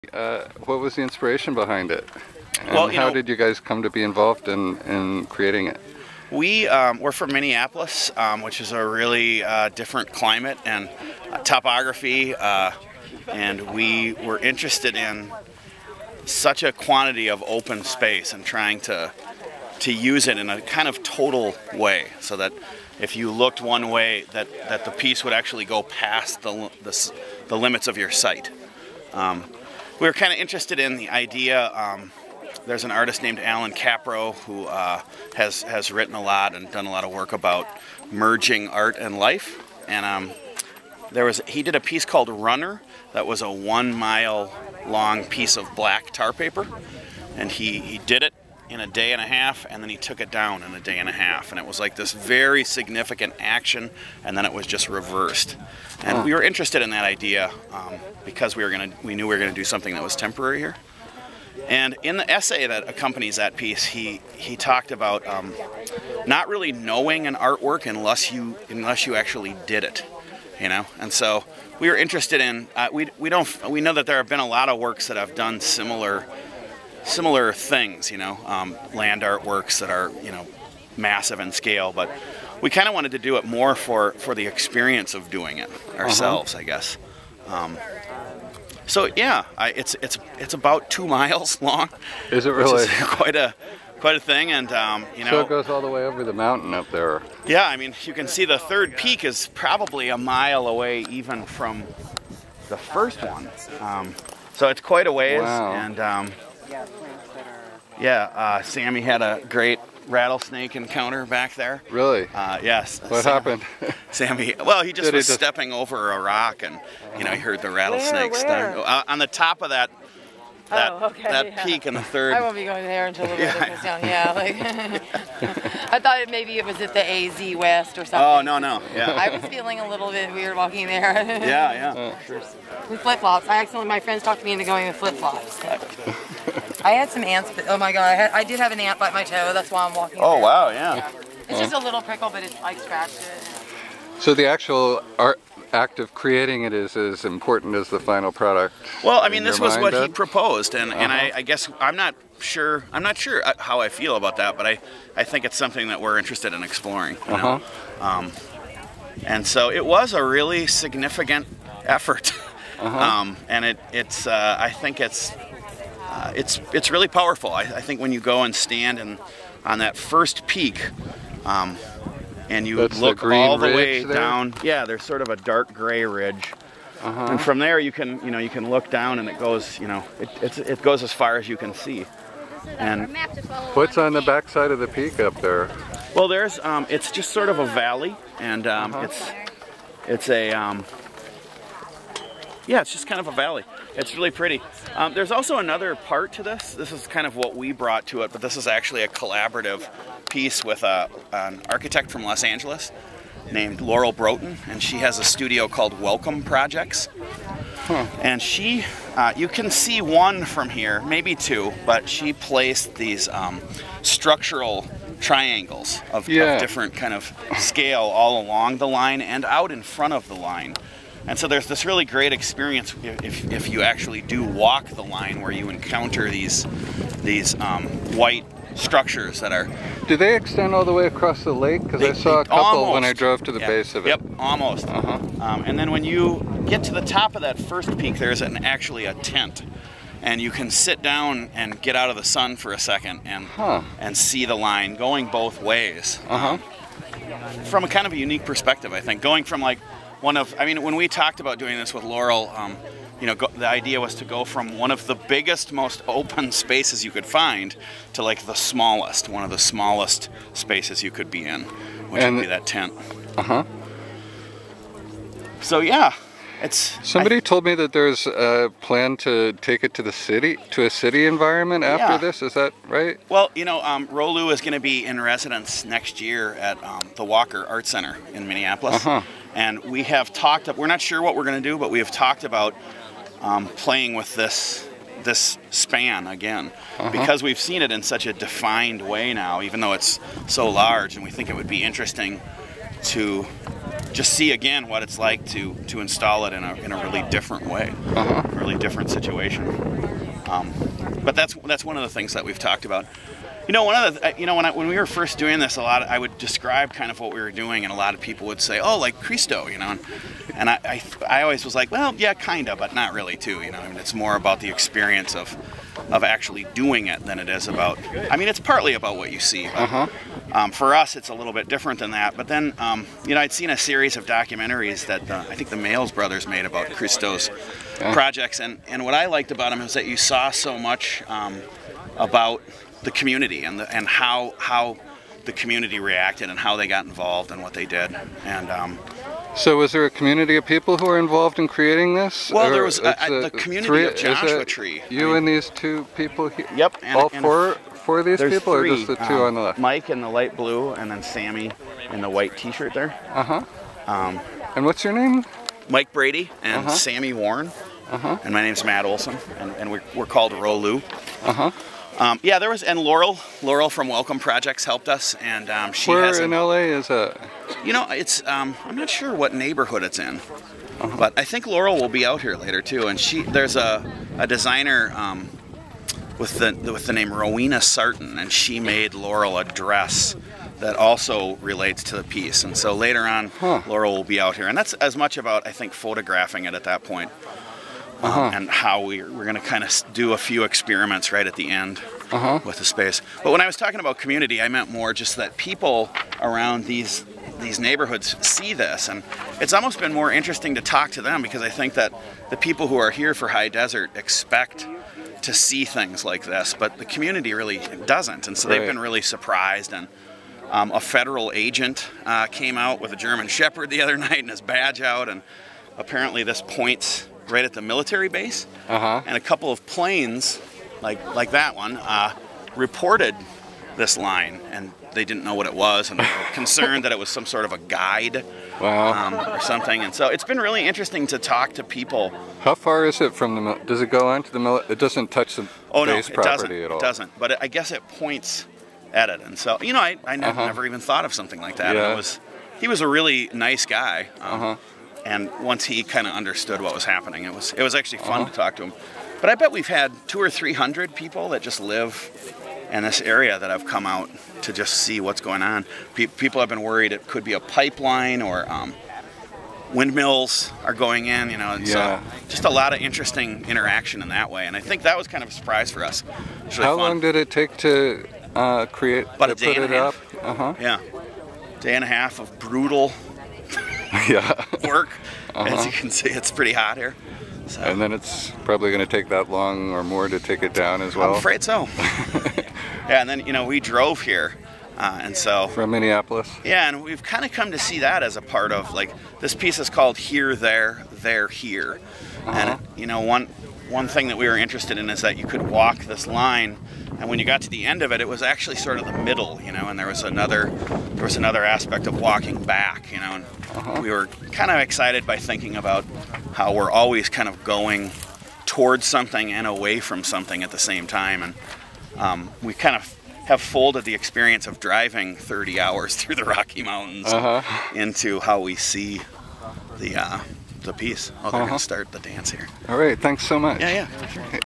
HDTS 2011 - Here There, There Here Matt Olsen ROLU Interview
Matt Olsen from ROLU discusses their 2 mile long fabric installation and triangular structures strewn from Coyote Dry Lake over the mountains north of the lake, created for High Desert Test Sites 2011 in collaboration with Laurel Broughton's Welcome Projects.